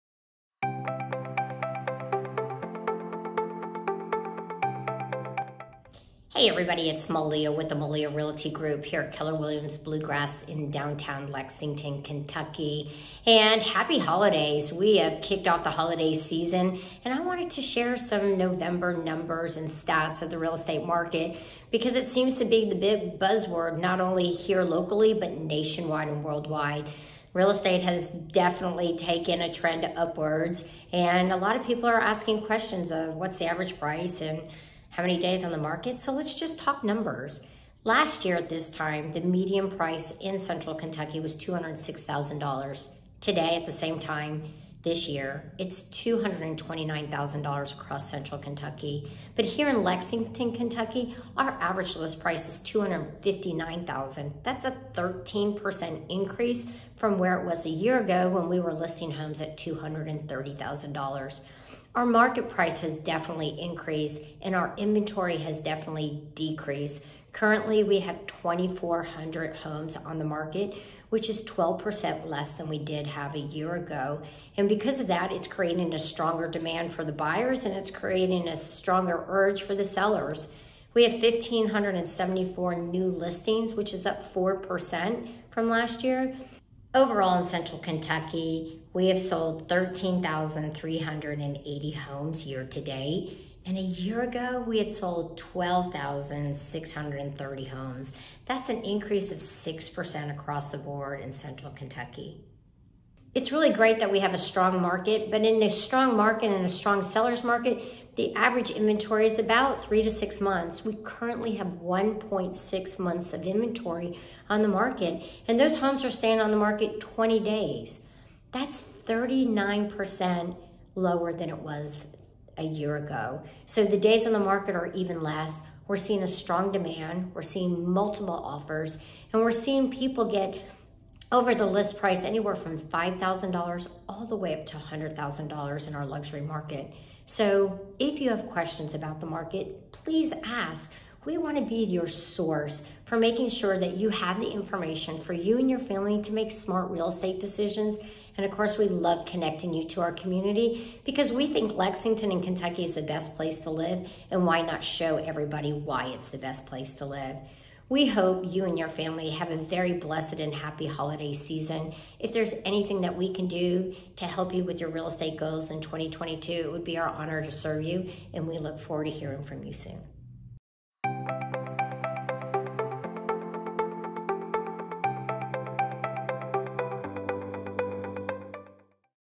Year-to-Date Real Estate Comparison
The holiday season is here. Real estate has really become quite the buzzword. We’re getting more people than ever who are interested in knowing the value of their homes. Today we want to compare last year’s market data to this year and give you an overview of how the market has fared. To see the full breakdown, watch this short video.

Melia Hord, CEO/Lead Agent
MELIA Realty Group
Keller Williams Bluegrass Realty

Looking to buy a home?

Looking to sell a home?